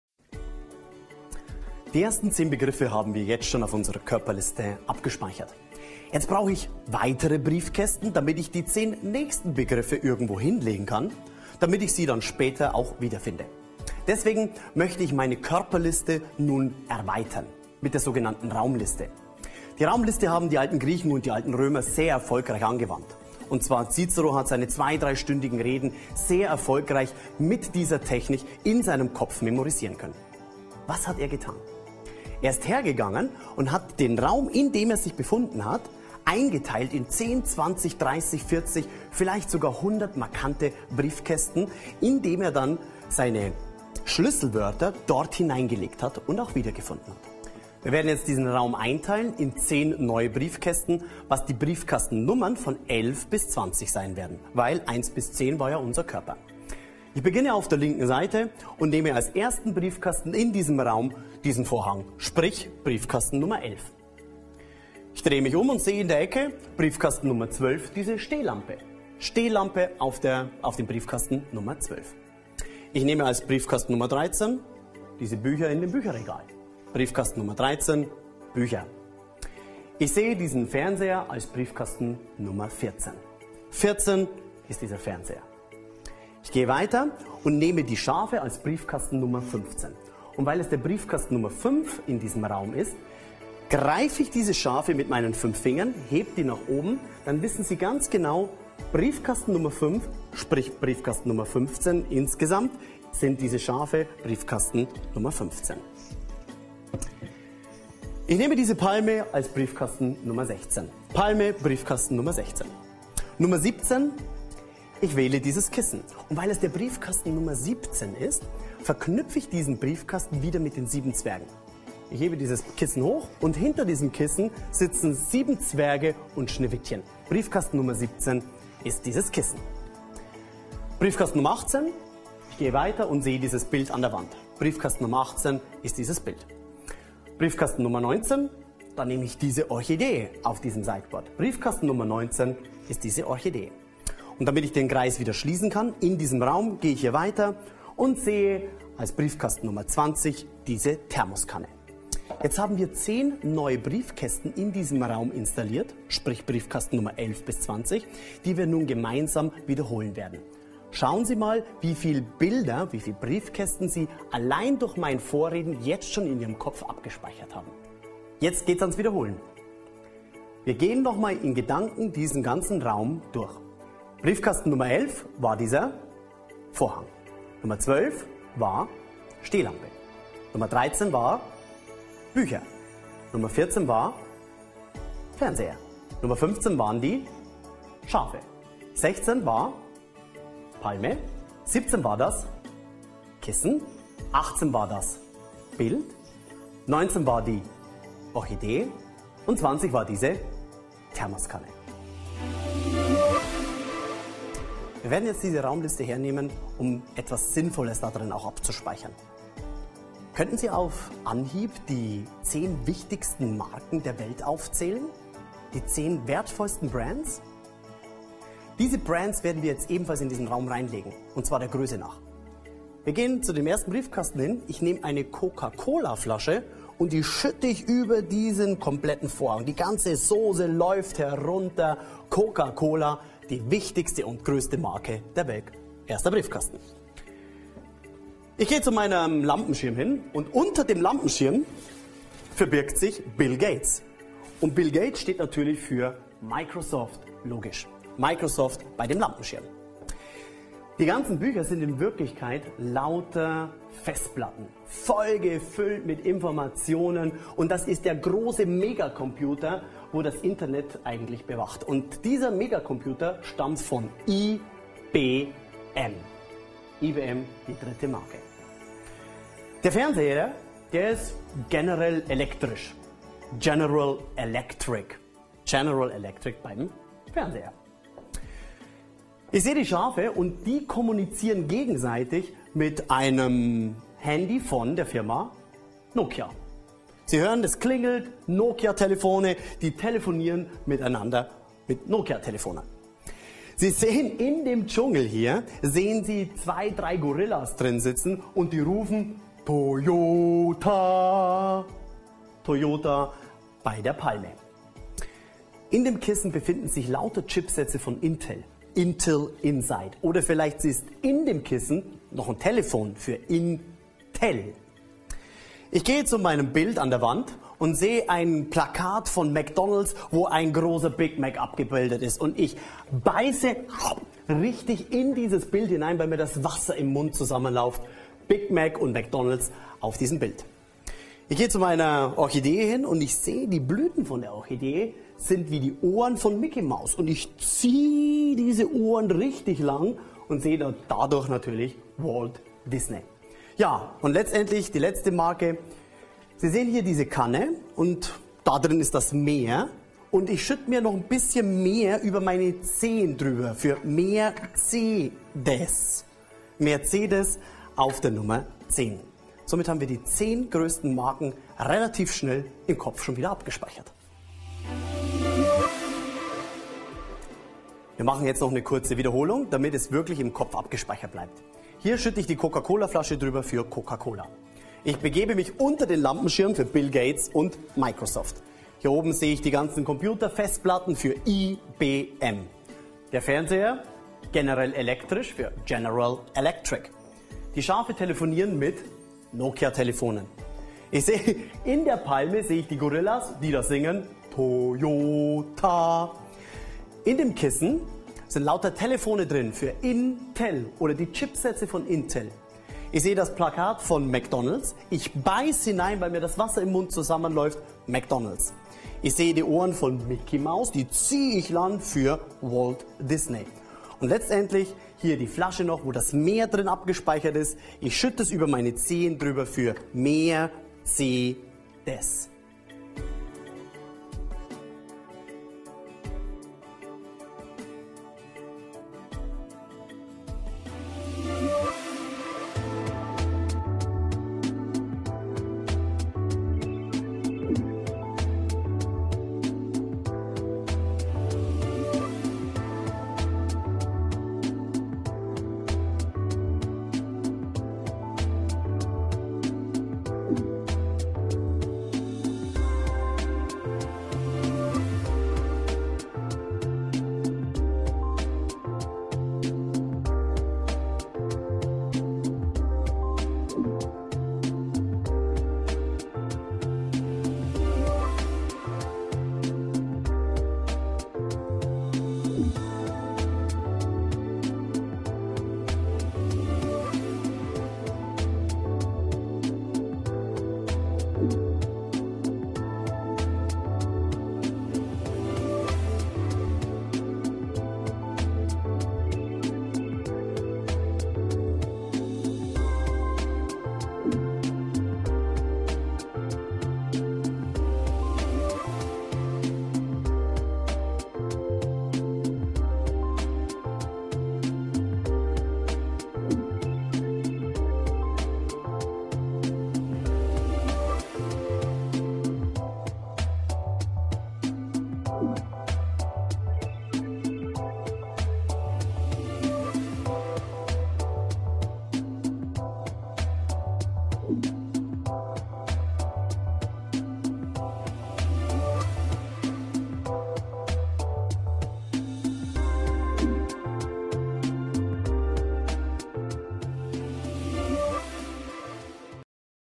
Dinge schneller merken mit der Raumliste - Markus Hofmann zeigt wie:
In diesem Video erwartet Sie:
- Loci-Technik -- wie installiere ich mentale Briefkästen?
- Die Raumliste, eine Erweiterung der Körperliste
- Lernen sie innerhalb kürzester Zeit die 10 wichtigsten Marken der Welt auswendig

Erlernen Sie mit Markus Hofmann, wie man sich schnell und spielerisch Wissen aneignet, damit Sie gelernte Lerninhalte sofort umsetzen können.

Hierbei bedient sich Markus Hofmann der alt-griechischen Mnemo-Technik, mit der die bildhafte Vorstellungskraft des Gehirns nutzbar gemacht wird. Das dies hervorragend funktioniert, merken Sie, wenn Sie gegen kleine Kinder Memory spielen. Vermutlich werden Sie keine Chance haben, da bei Ihren Gegnern jene bildhafte Art des Denkens immer noch funktioniert. Erst später geht diese Eigenschaft allmählich verloren, wenn sich unsere zwei Gehirnhälften auf unterschiedliche Fähigkeiten spezialisieren.

Doch verzagen Sie nicht, denn Sie müssen diese strikte Trennung keinesfalls so hinnehmen! Markus Hofmann wird Ihnen zeigen, wie Sie wieder beide Hälften zum Denken aktivieren, sodass Sie sich beispielsweise Namen und Zahlen leichter merken, oder Fremdsprachen besser lernen können.

Sehen Sie sich also folgendes Video an und machen Sie den ersten Schritt, ein umfassend wissender Mensch zu werden.